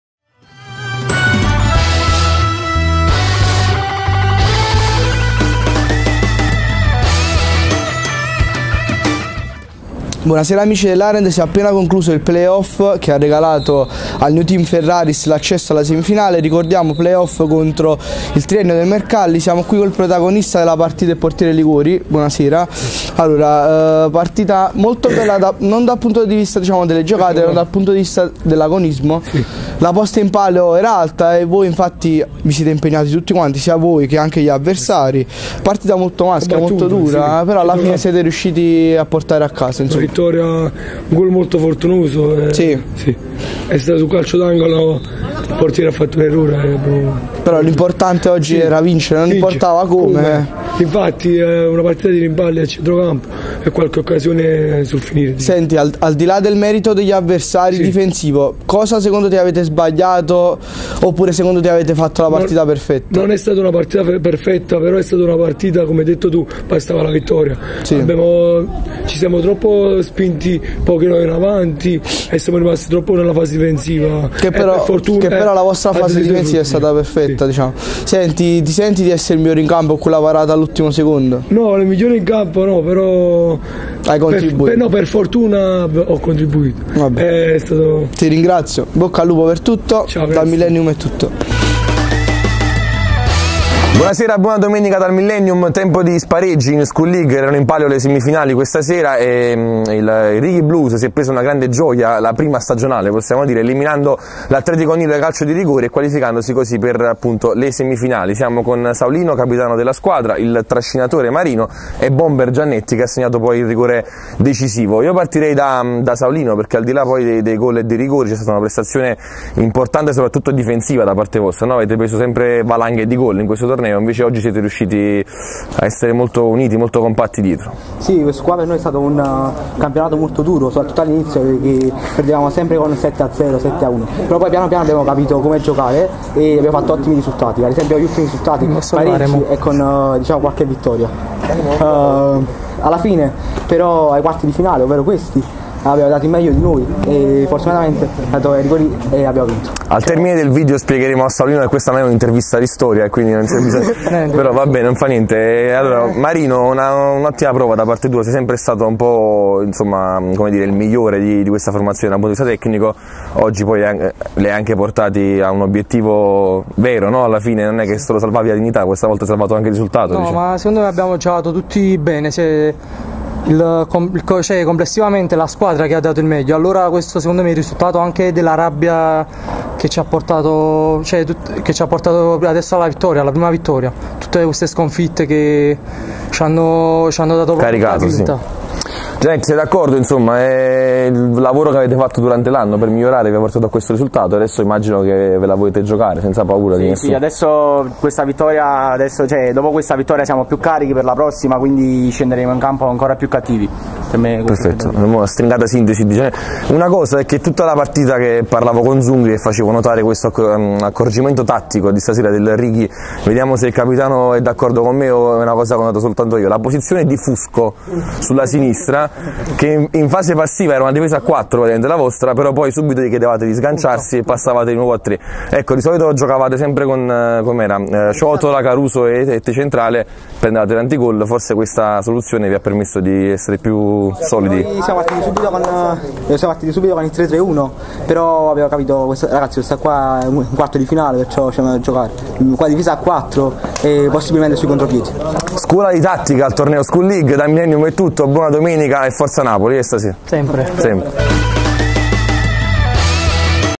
Interviste Playoff | SCHOOL LEAGUE C8 1°edizione
Sono intervenuti ai microfoni dei giornalisti della Asd Arend:
Armando Liguori (New Team) ed anche Vincenzo Giannetti, Alessio Marino e Giuseppe Saulino (Blues)